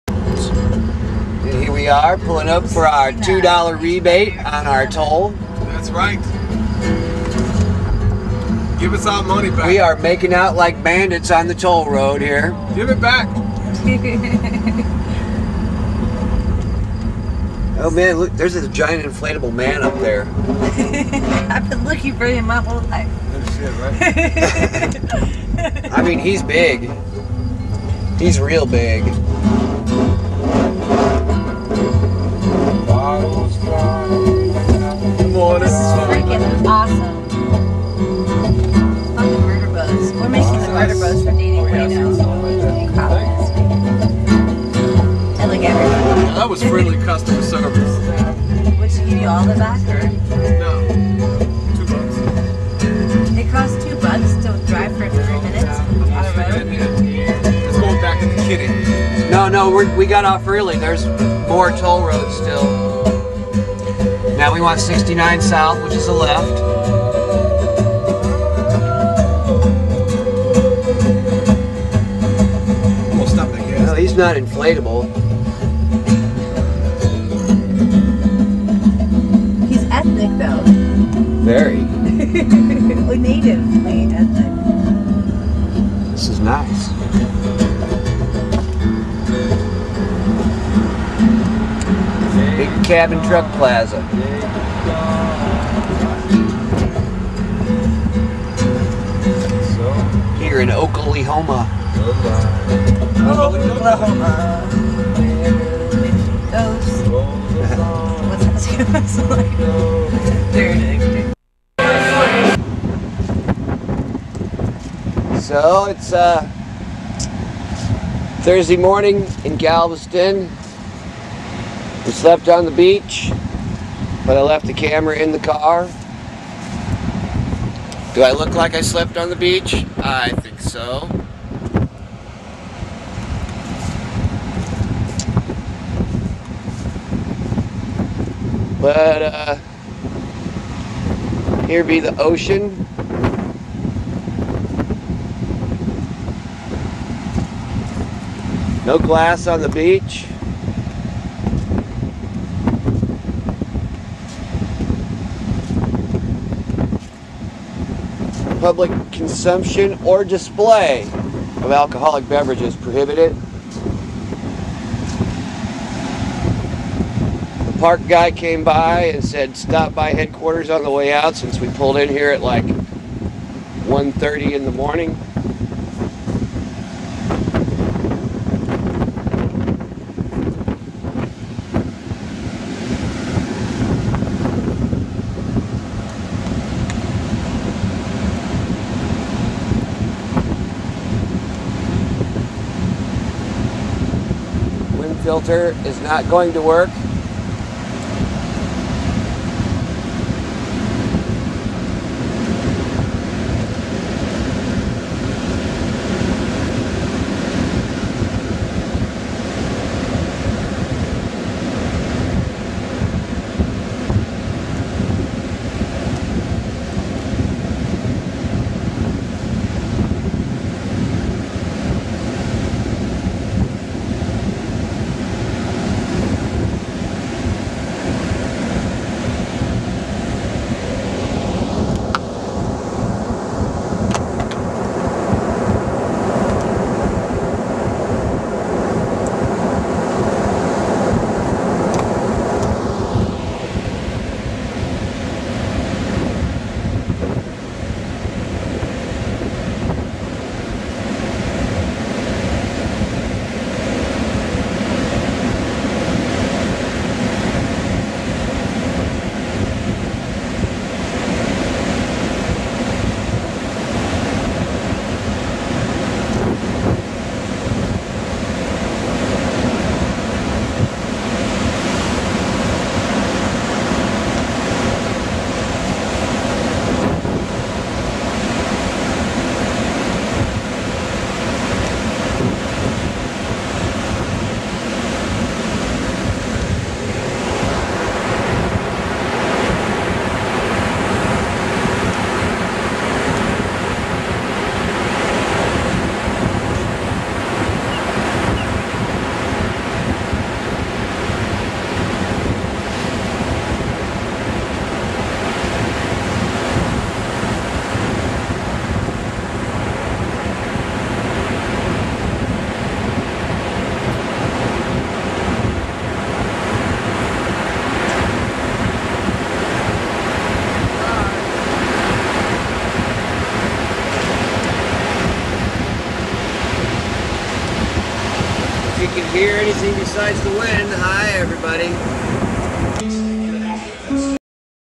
juxtaposition of tollbooth and galveston beach
some random video off my camcorder starting with getting our "rebate" at the oklahoma tollway station around 1am on the way to houston, and ending with waking up on galveston beach looking a bit more scruffy than usual, though not as scruffy as I would by the end of the friends and family tour